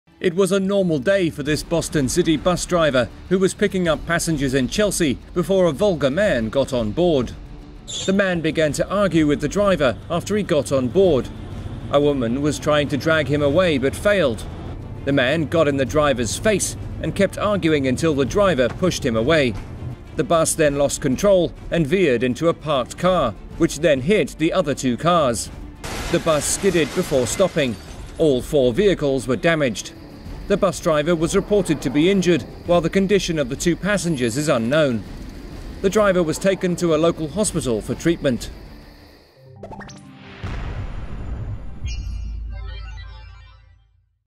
Boston MBTA bus crashes into 3 cars after man assaults driver
Boston MBTA bus crash: dramatic video released by Transit Police shows a male passenger assaulting a bus driver, resulting in his bus crashing into three parked cars. The assault and accident took place around 9:30pm on Sunday in the Chelsea neighborhood of Boston.

In the video, the male passenger can be seen boarding the bus with a female companion. The man attempts to pay and some sort of verbal argument ensues. The woman seen in the video attempts to pull the argumentative man toward the back of the bus.

But the man got into the driver's face and kept shouting at him until he was pushed away by the driver with force. This is all we see from inside the bus. There is more footage, but Transit Police have refused to release it, in order to protect the driver's identity.

An external camera on the bus then shows the bus crashing into a parked car. That parked car then smashes into two other cars parked in front of it.

The bus driver was injured in the assault and accident. Meanwhile, the conditions of the two passengers, along with their identities, remain unknown.

The one-minute video is posted on the police department's blog and police are asking witnesses to identify the man.